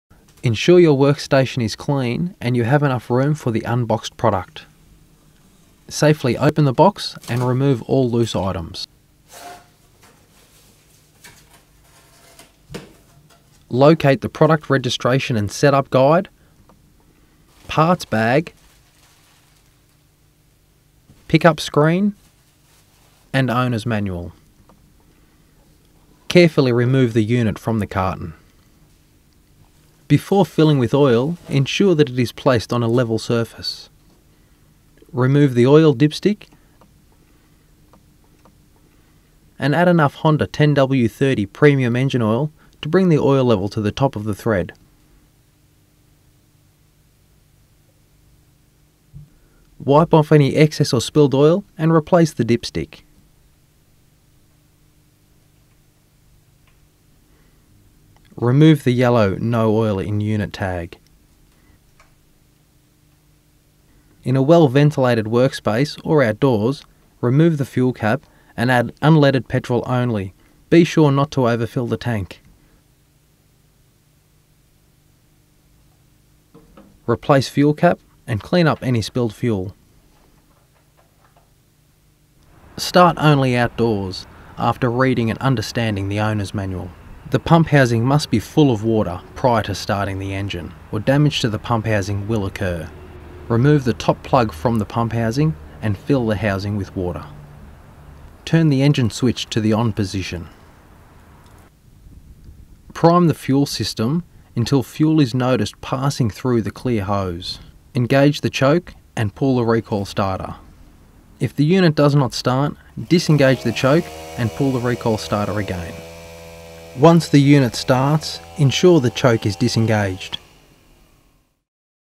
Honda WX10 Water Pump Set Up
This video outlines the correct method to set up  the WX10 Water Pump.

To find our more information on Honda 10W-30 Premium Engine Oil and chemicals